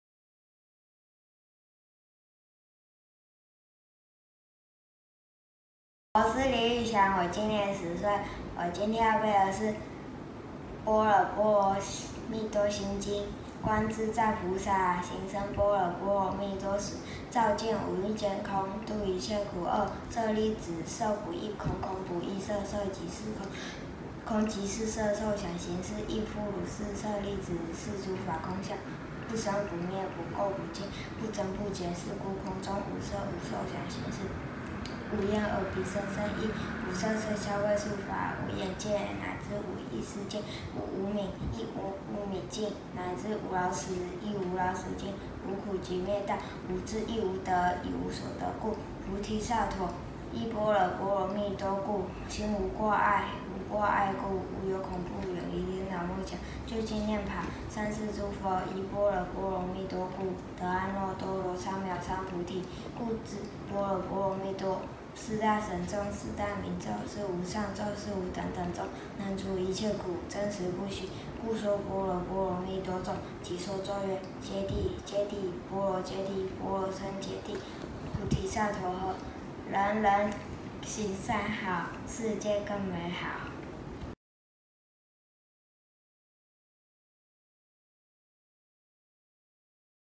三好兒童快樂讀經暨說故事影片徵選 【第457集】
佛光山南台別院 / 教師分會 / 讀經組  / 李**
主題：般若波羅蜜多心經

【請鎖定】
國際佛光會中華總會/ Facebook紛絲專頁：